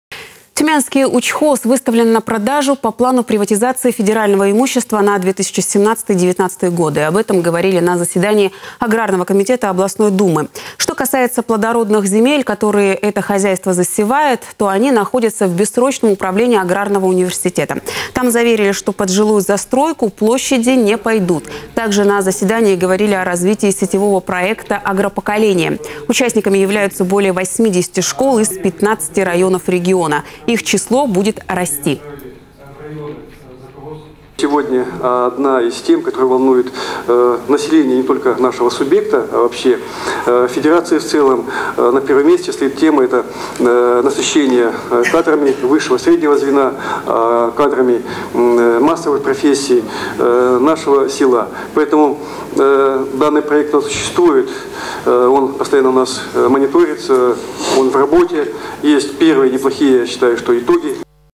Тюменский Учхоз выставлен на продажу
Тюменский Учхоз выставлен на продажу по плану приватизации федерального имущества на 2017-19 года. Об этом говорили на заседании аграрного комитета областной думы. Что касается плодородных земель, которые это хозяйство засевает, то они находятся в бессрочном управлении аграрного университета. Там заверили, что под жилую застройку площади не пойдут. Также на заседании говорили о развитии сетевого проекта «Агропоколение». Участниками являются более 80 школ в 15 районах региона. Их число будет расти.